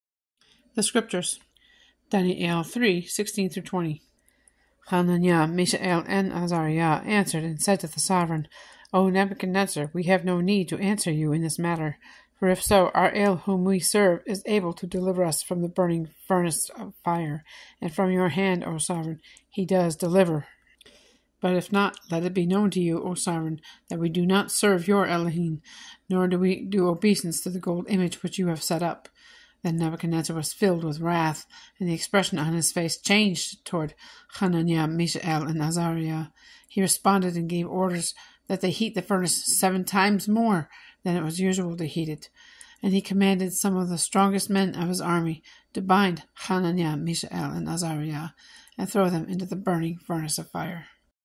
Dani'Ĕl 3:16-20. Nebuchadnezzar's image of gold, part 5 of 8.
the testing of HananYah, Misha'Ĕl, and AzarYah